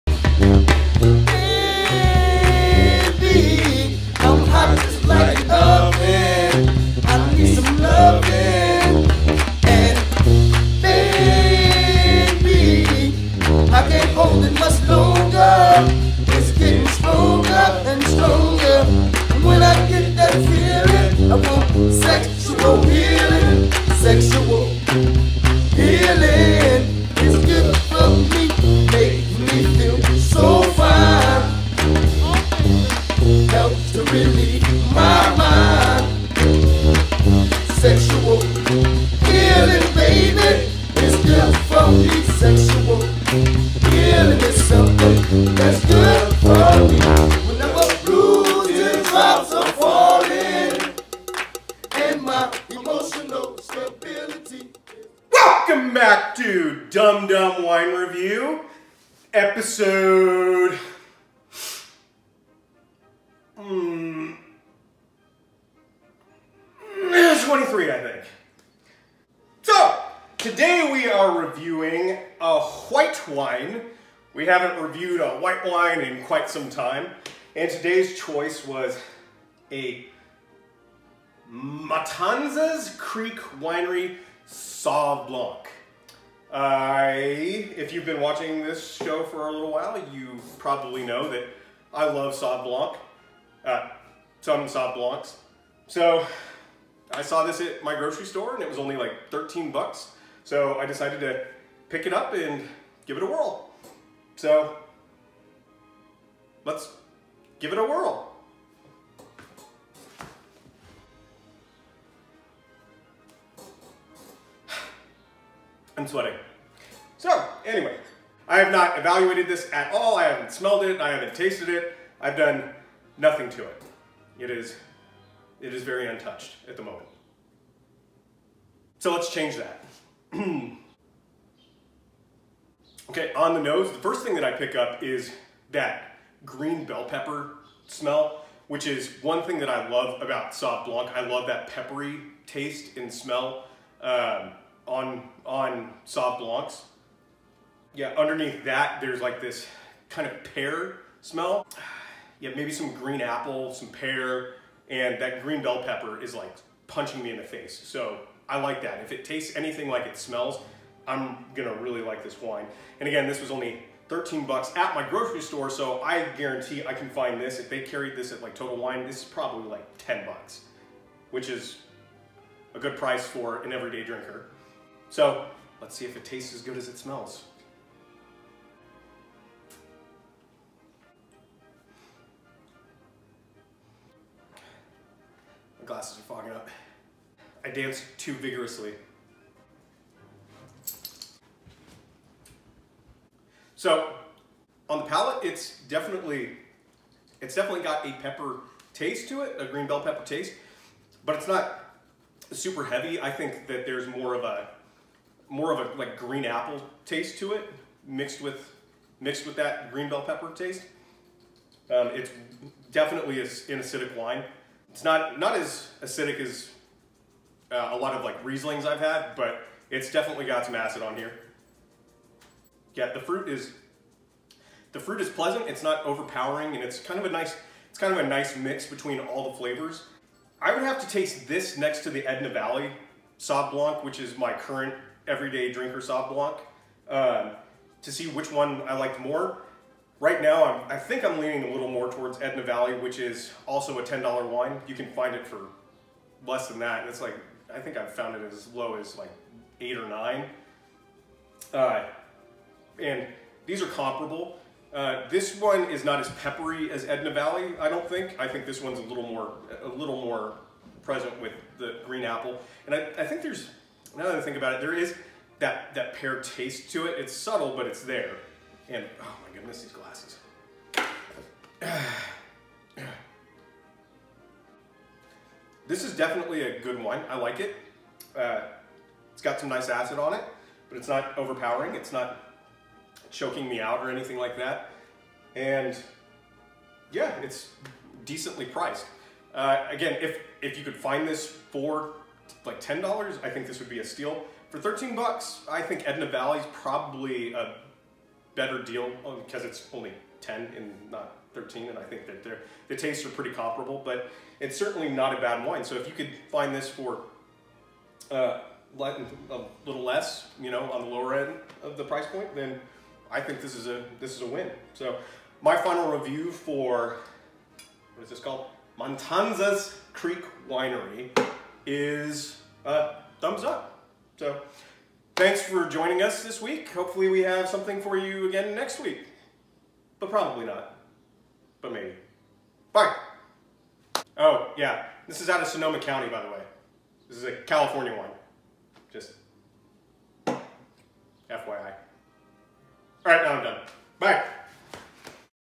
DumbDumb Wine Review: A Simple Review From a Simple Man Ep. 23 Matanzas Creek Winery Sauvignon Blanc
Ep. 23 Matanzas Creek Winery Sauvignon Blanc